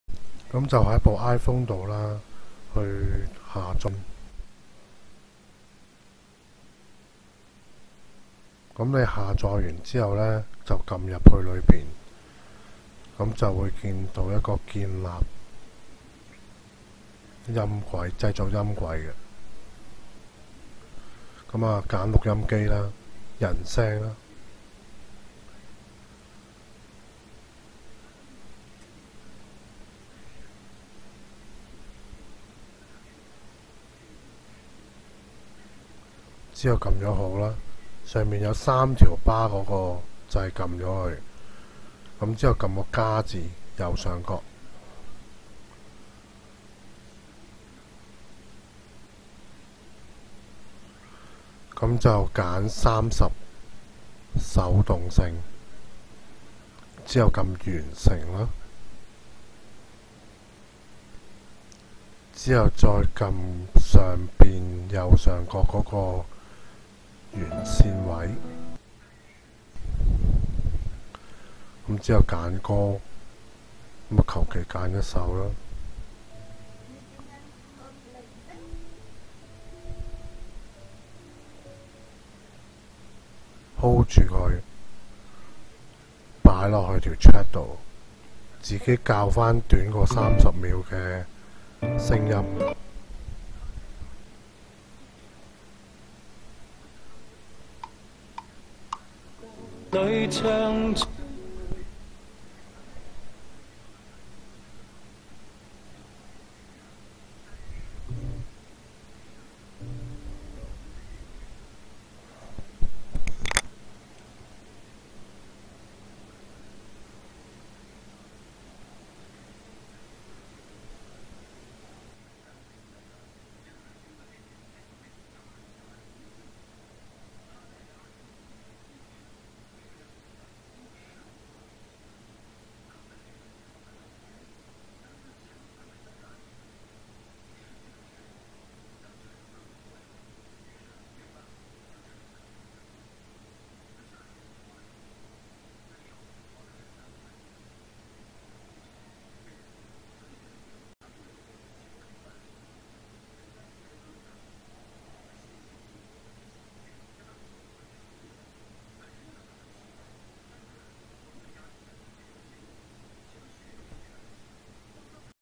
iPhone 製作電話鈴聲 冷知識粤语
如何不用電腦在 iPhone 上做出手機鈴聲 粤语